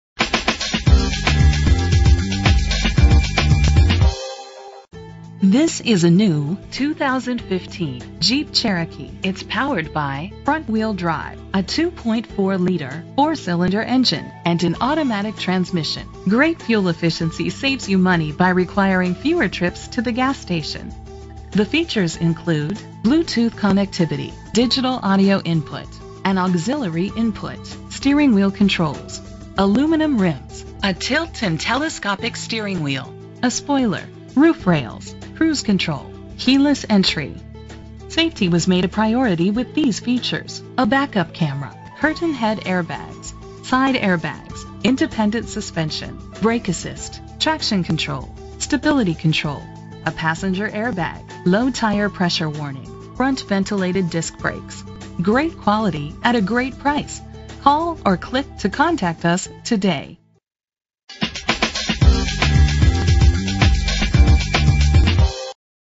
2015 Jeep Cherokee - Columbus OH
Year: 2015
Make: Jeep
Model: Cherokee
Trim: Latitude
Engine: 2.4 liter inline 4 cylinder 16 valve
Transmission: 9-Speed Automatic
Color: Billet Silver Clearcoat Metallic
Mileage: 0
Address:
6350 Perimeter Loop Rd